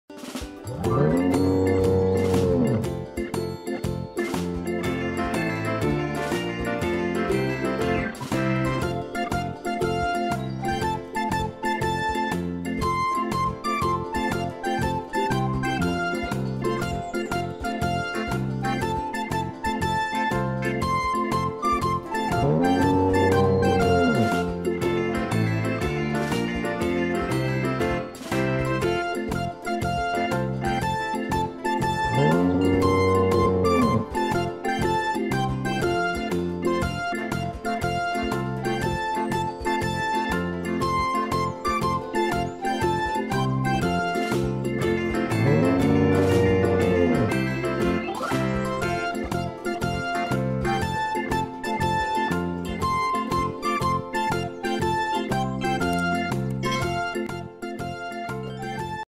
PLOWING, Ploughing using ox toy stop motion | Toy animals | animais de boi |wethinkio farm
Plowing, Ploughing using ox toy stop motion.

HAVE FUN.!
We Hope You Enjoy the video.